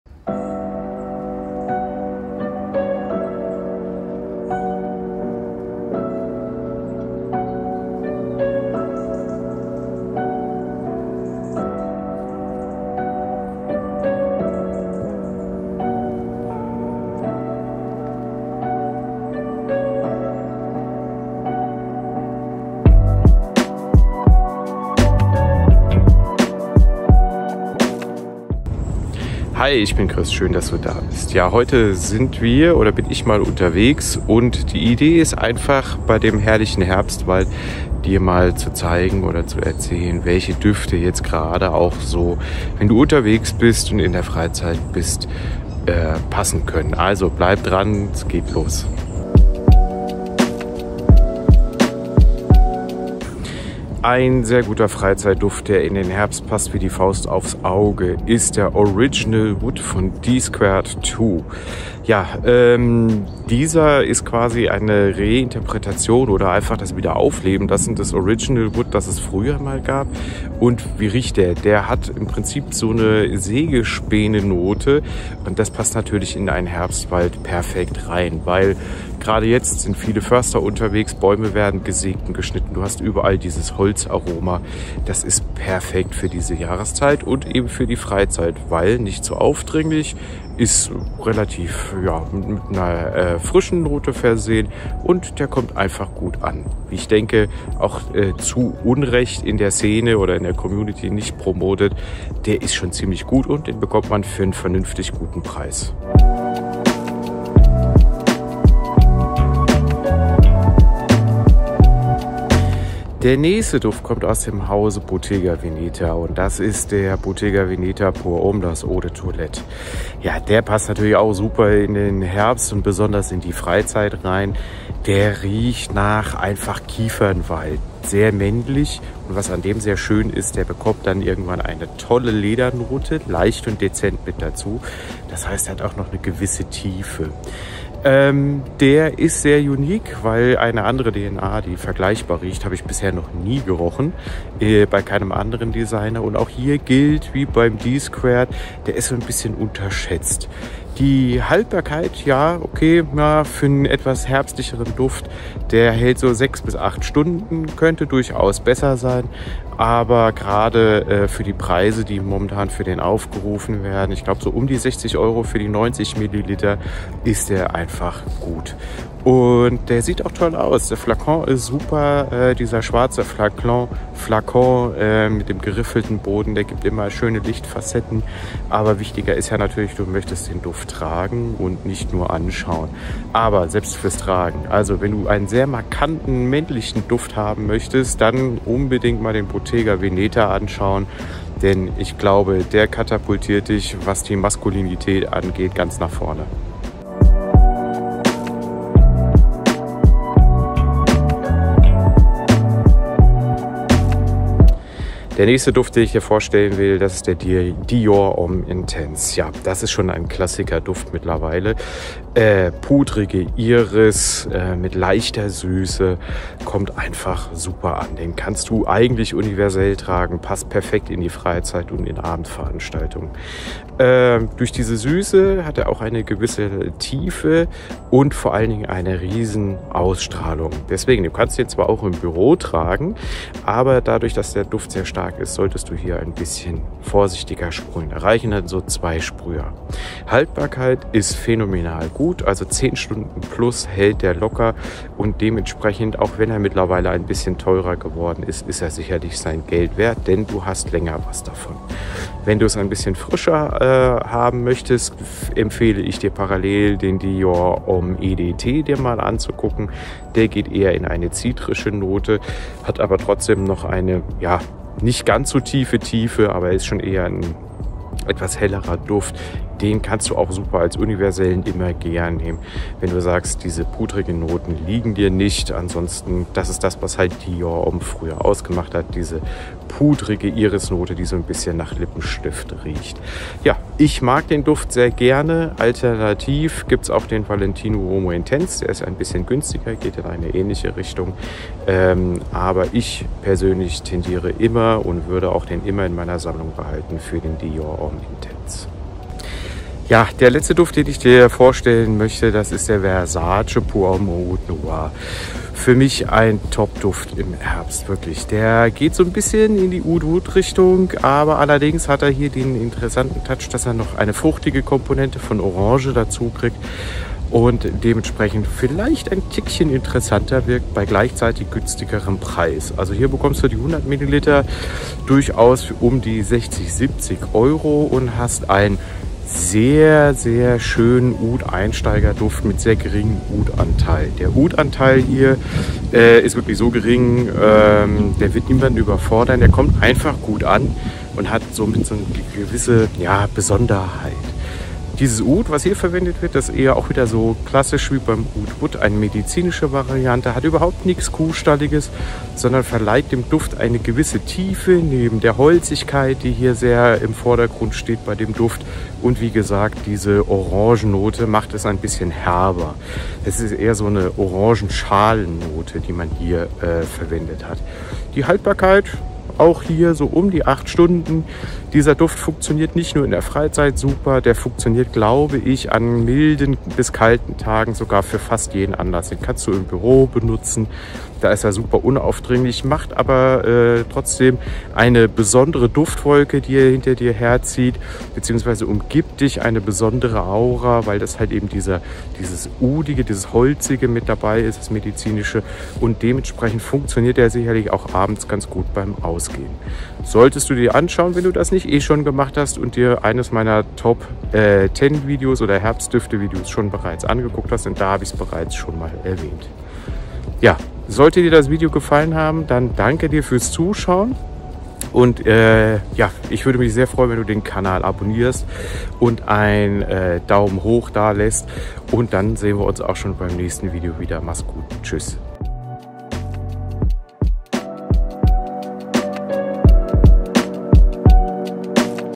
TOP DÜFTE | Für die FREIZEIT - Herbst Edition
Heute zeige ich DIR TOP Düfte für die Freizeit, die Du im Herbst tragen kannst. Lass dich inspirieren.

[WERBUNG]
Du möchtest einen der gezeigten Düfte testen? Kein Problem. vorbei und sichere Dir eine Abfüllung.
Damit kannst Du zielgerichtet deinen nächsten Traumduft finden, ohne dein Geld in falsche Düfte zu investieren.

Equipment:
Kamera: Panasonic S5
Beleuchtung: Godox SL 60W
Mikrofon: ZOOM Field Recorder F2